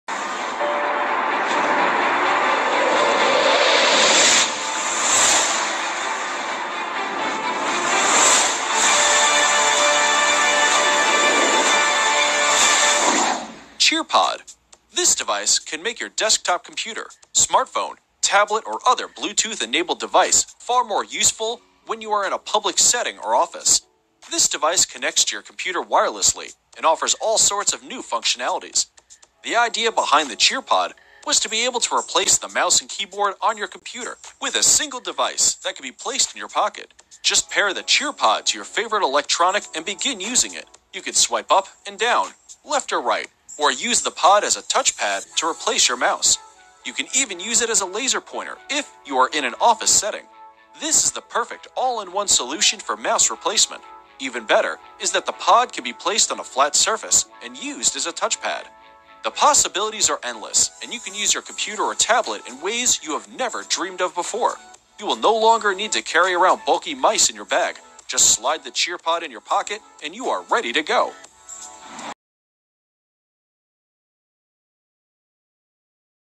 CheerPod Smart TouchPad
As a game-changer, CheerPod can be used as a full-function desktop mouse, touchpad and laser pointer on virtually any device. With support for the most frequently used hand gestures in both Desktop Mode and Air Mode, it’s comfortable to use and easy to control. No matter whether you are working in the office, giving a presentation, or enjoying some entertainment on your sofa, CheerPod is the perfect productivity tool and control device for multi-device!